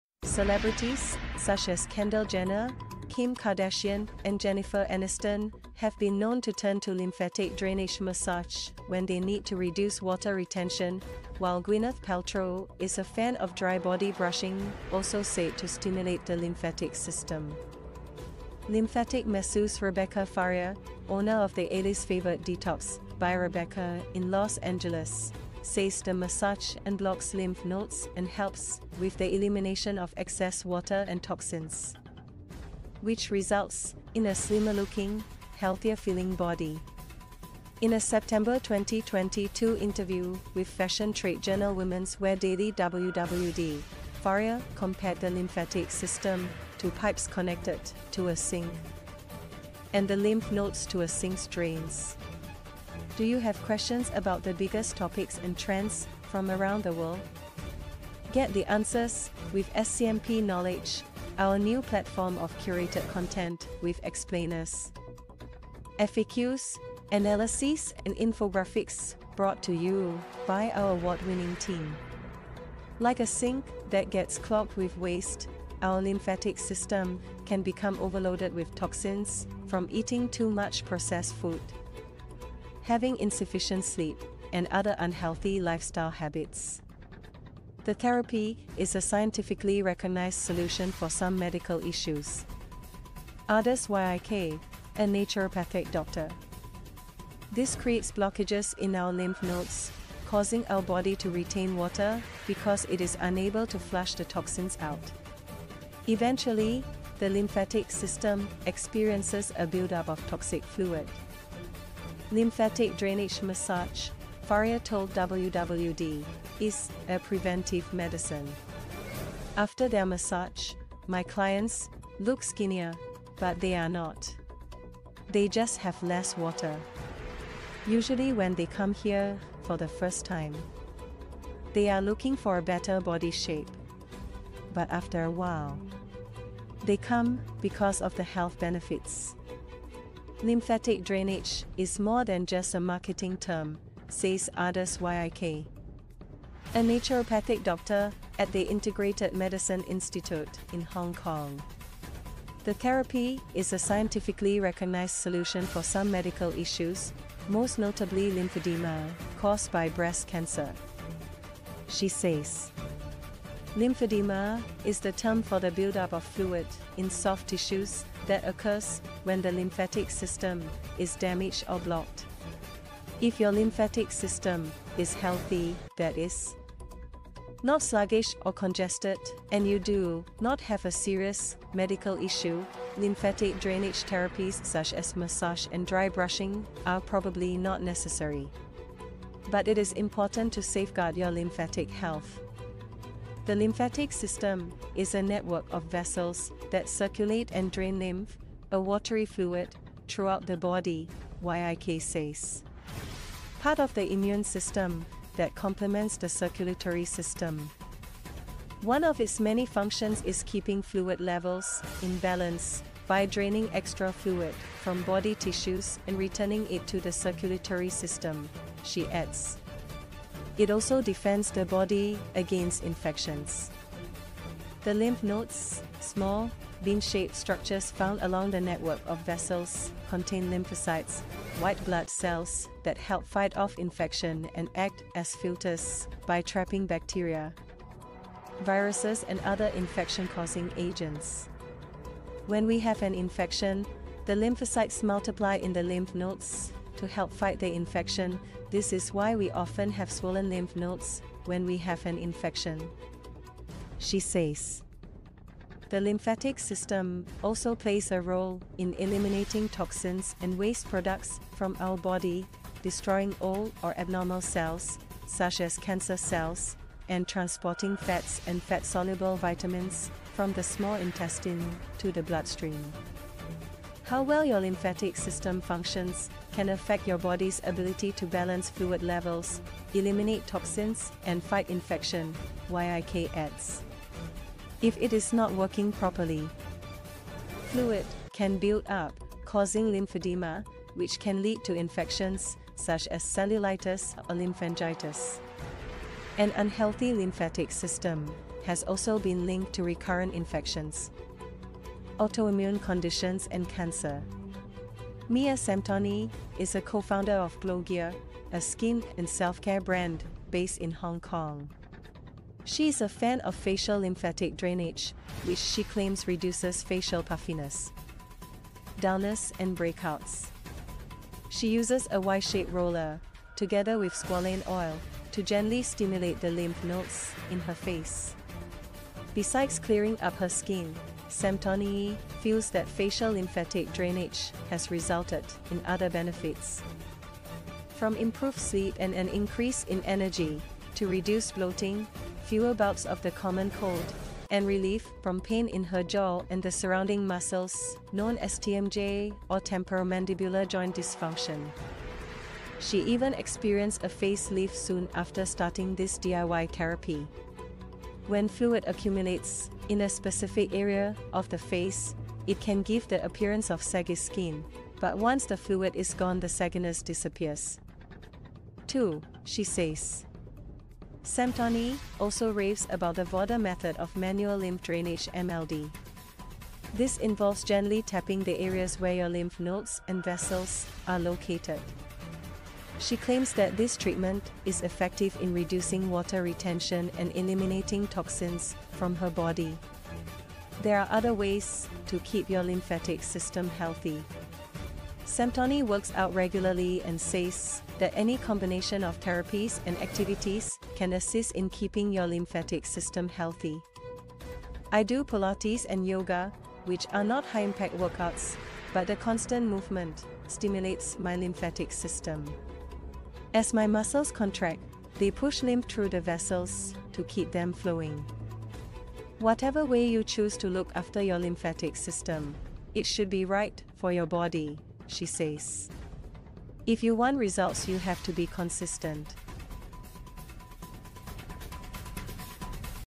Why Lymphatic Drainage Massages Have Fans in Kendall Jenner, Kim Kardashian, and Jennifer Aniston
"🆈🅾🆄🆃🆄🅱🅴: - Why Lymphatic Drainage Massages Have Fans in Kendall Jenner, Kim Kardashian, and Jennifer Aniston: A Healthy Lymphatic System Detoxes and Makes You Look Slimmer

- Lymphatic drainage massages have gained popularity among celebrities like Kendall Jenner, Kim Kardashian, and Jennifer Aniston.
- The lymphatic system plays a crucial role in detoxifying the body and maintaining a healthy immune system.
- Lymphatic drainage massages help stimulate the lymphatic system, improving circulation and removing toxins from the body.
- These massages can also help reduce water retention and bloating, leading to a slimmer appearance.
- Regular lymphatic drainage massages may have additional benefits such as reducing cellulite and improving skin tone.